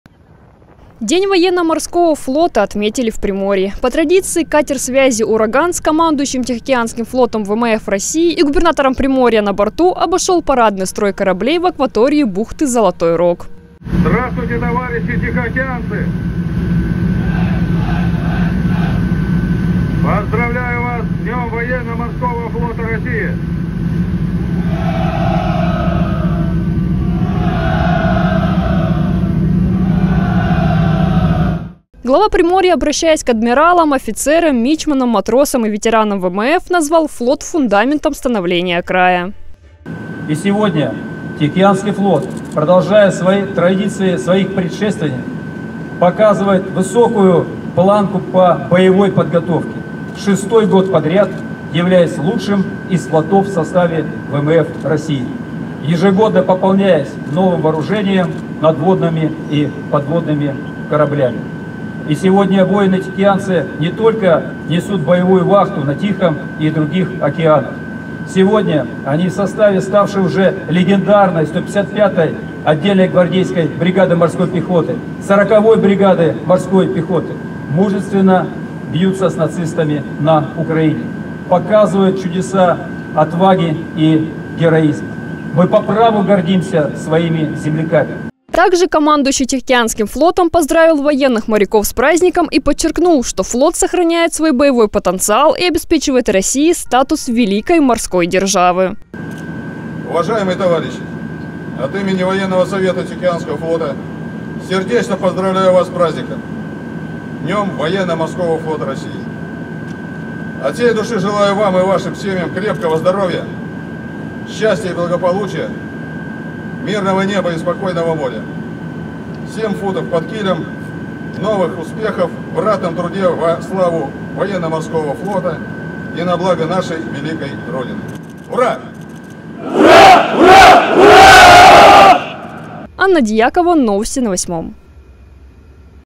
Глава Приморья поздравил военных моряков-тихоокеанцев с Днем ВМФ.
День Военно-Морского флота отметили в Приморье. По традиции катер связи «Ураган» с командующим Тихоокеанским флотом ВМФ России и Губернатором Приморья на борту обошел парадный строй кораблей в акватории бухты Золотой Рог.